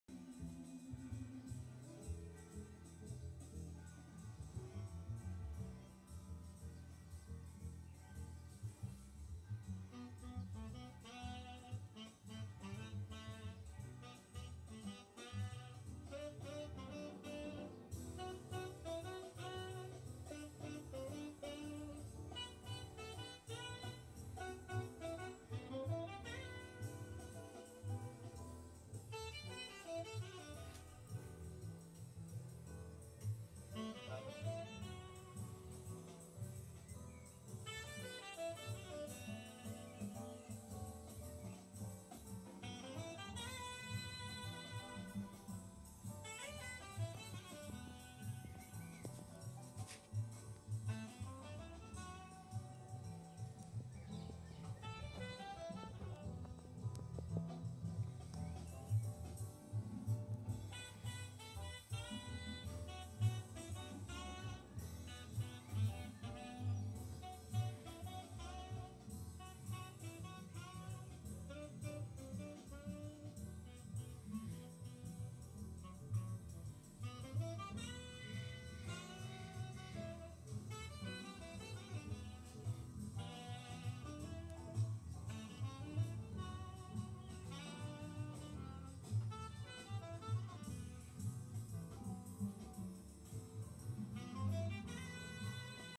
typographer's drawer with mosaics.butterflies and raw glass details
I show you my artistic works, I love to give new life to old typographer's drawers or worn boards, I use glass, hand-colored tiles and other mixed techniques